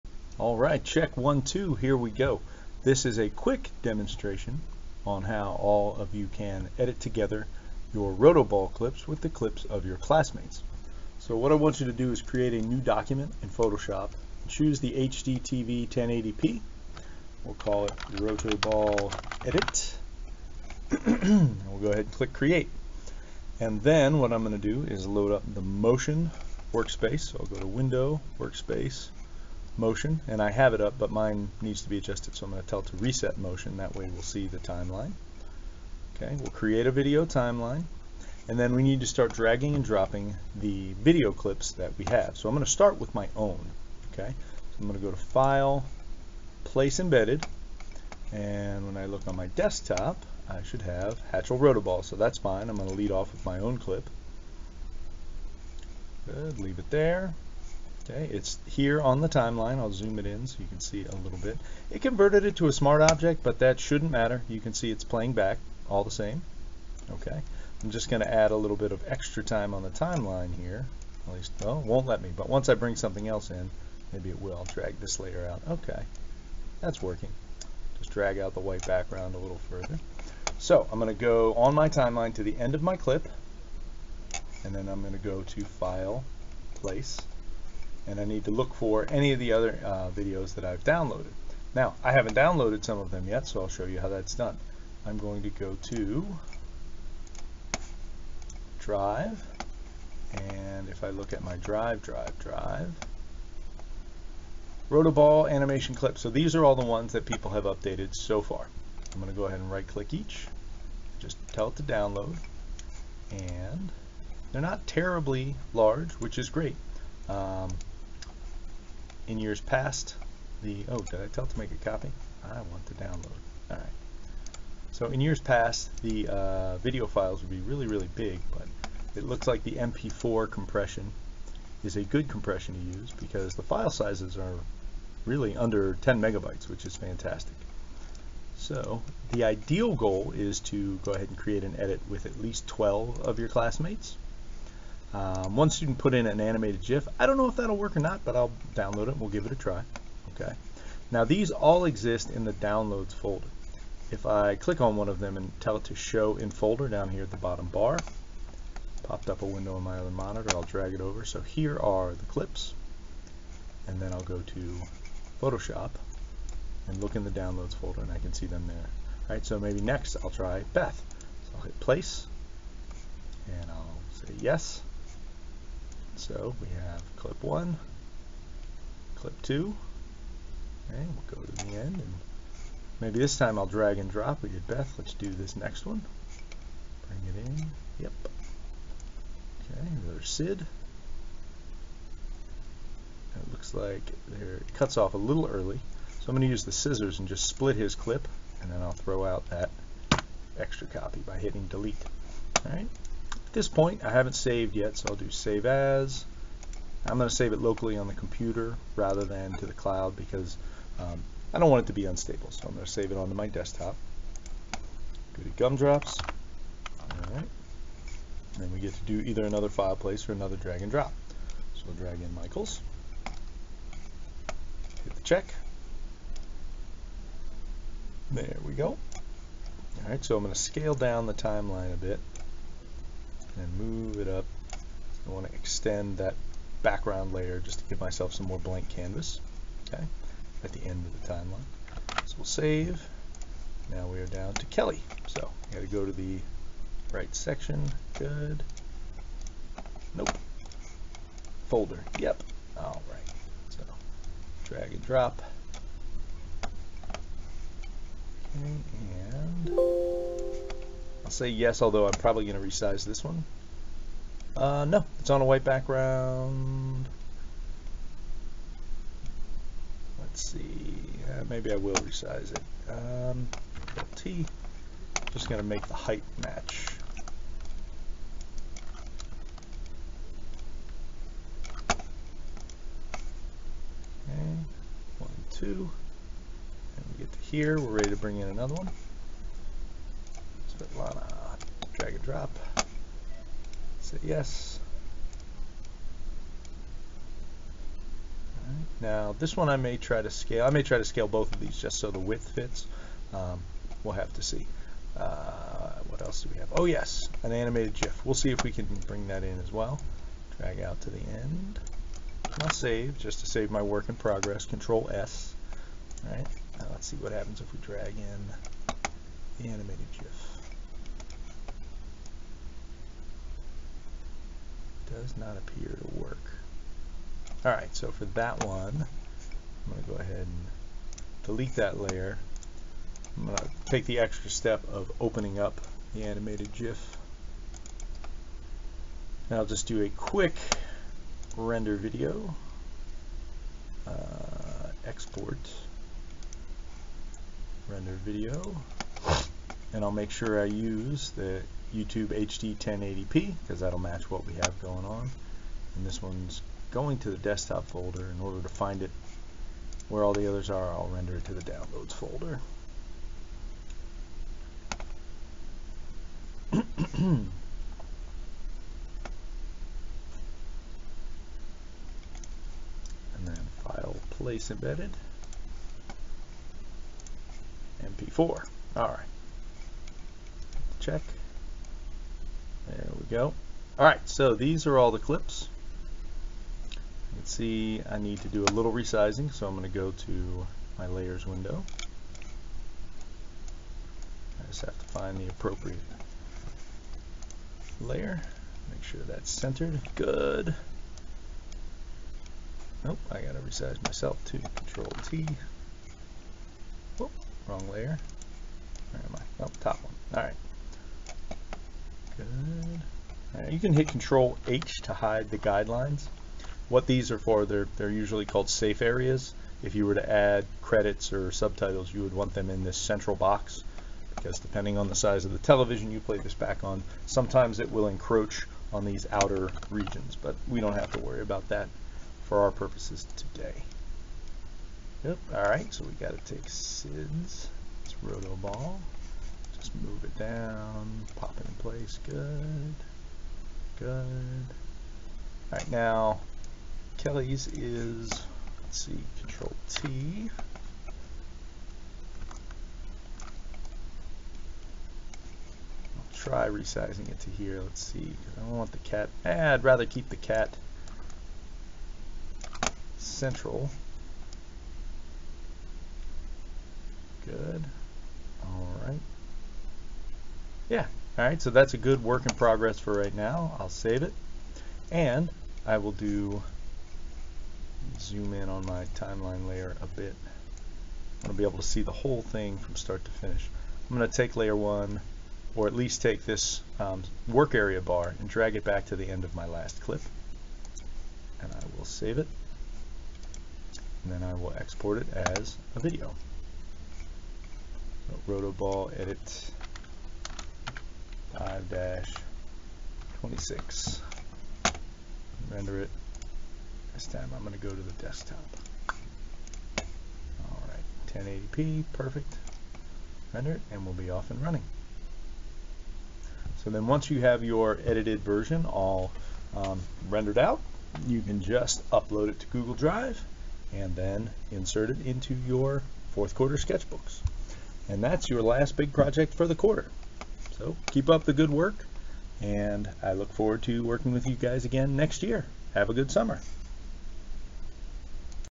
Rotoball Edit Demo
Mr. Hatchl demonstrates how to edit video clips together in Photoshop.